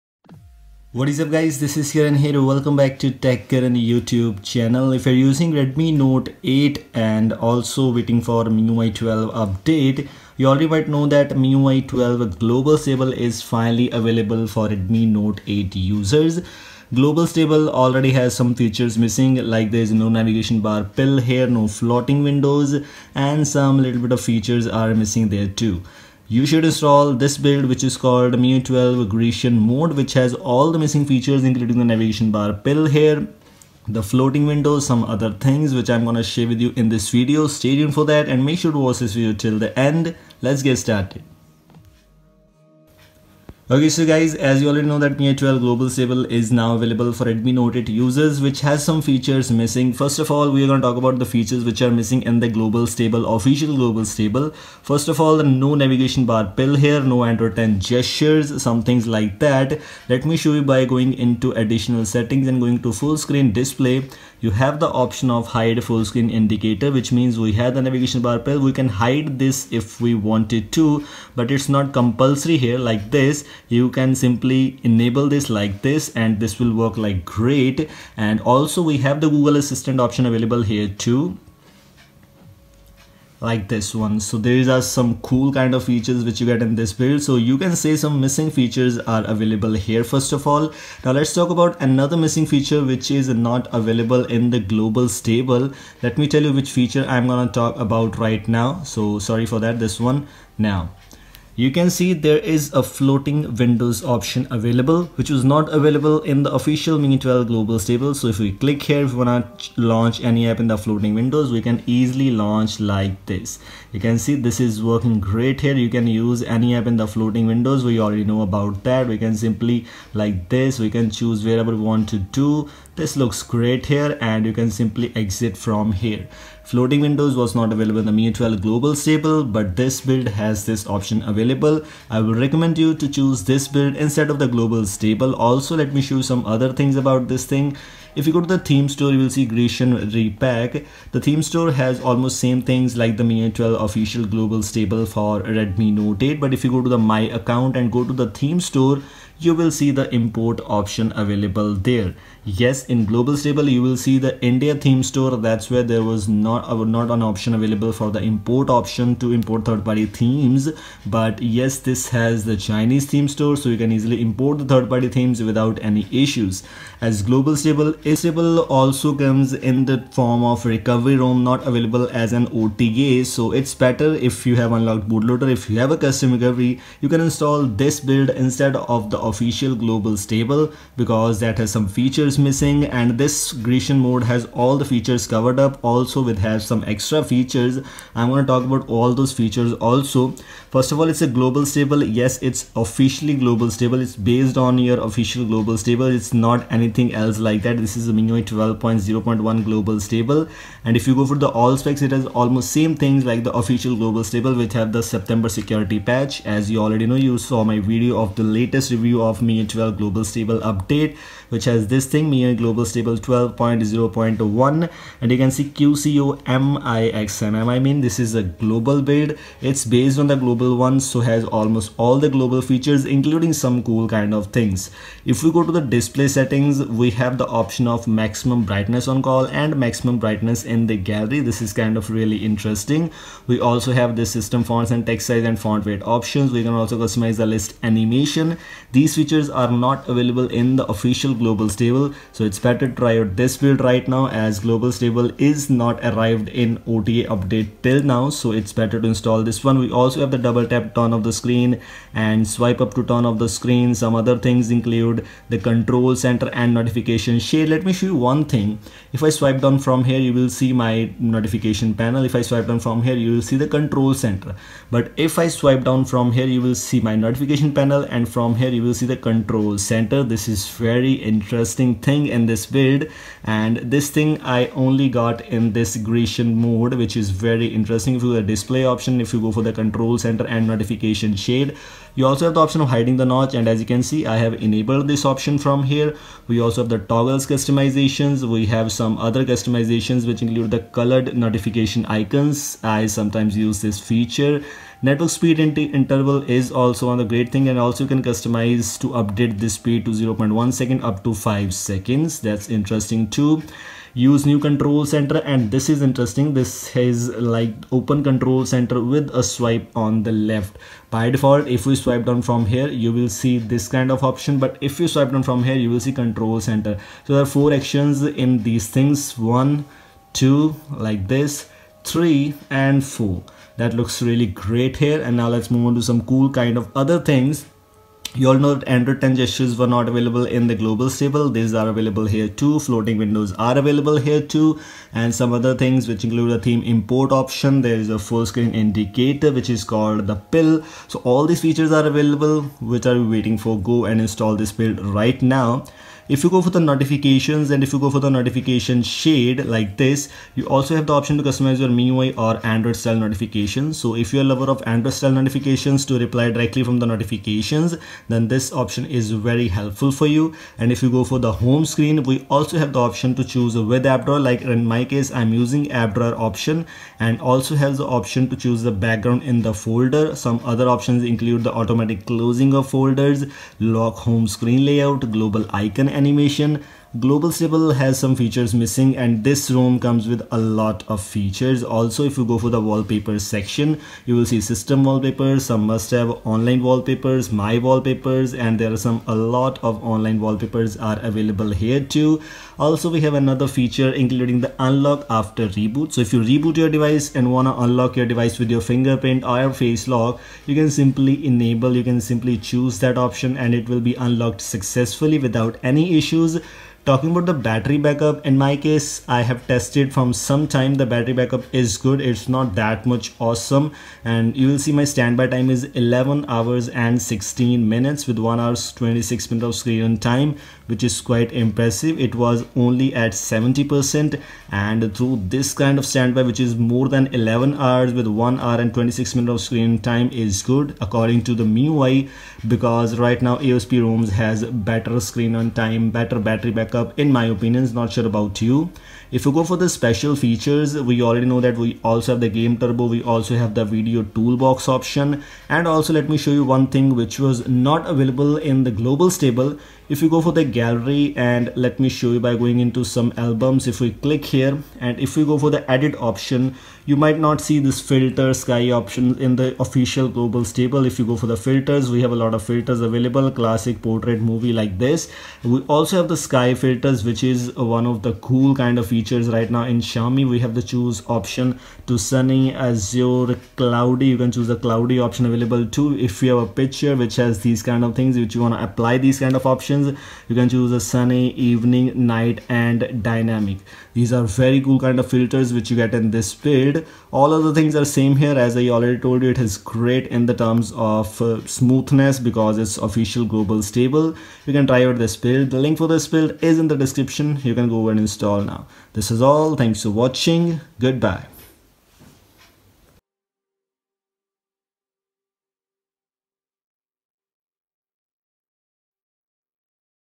MIUI 12 - Greeshan Mod for REDMI NOTE 8 | Use Missing Features like A10 Gestures, Floating Windows🔥🔥
Hey guys, What's Up? Everything good I Hope. In this Video, I am gonna show you "MIUI 12 - Greeshan Mod for REDMI NOTE 8 | Use Missing Features like A10 Gestures, Floating Windows🔥🔥" Make Sure to watch this video till the end to understand everything.

How to Install & Uninstall Magisk on your Android?